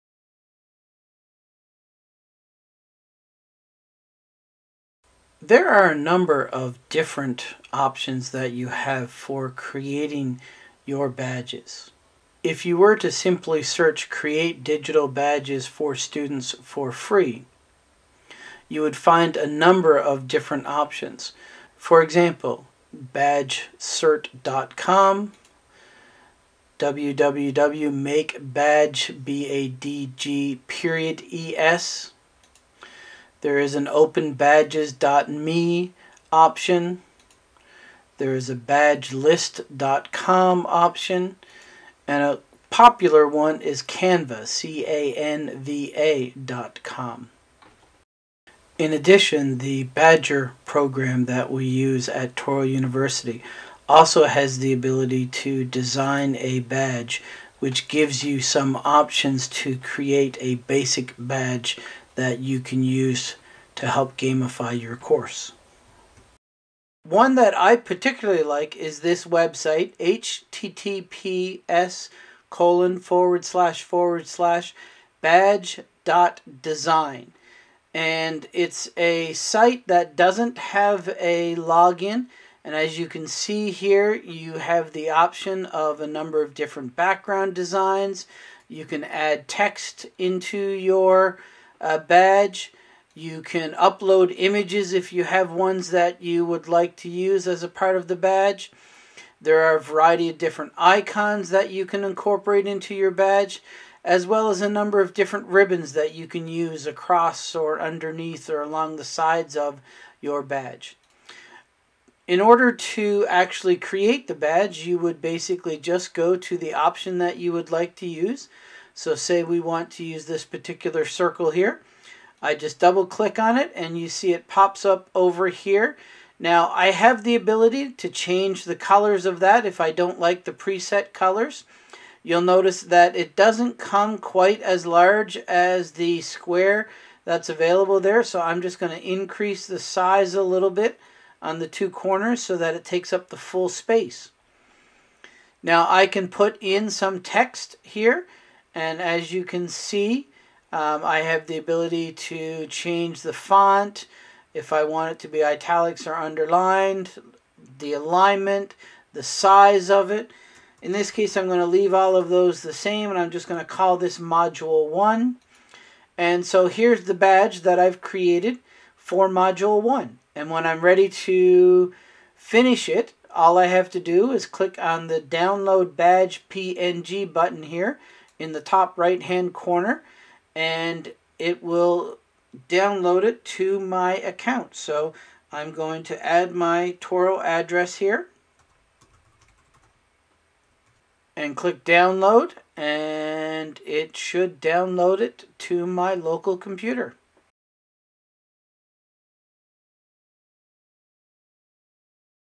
MicroCredentials@Touro: How to Create a Badge
This 3-minute video describes different programs that can be used to create a badge.  As a part of the video, it specifically demonstrates how to create and download a badge using

MicroCredentials@Touro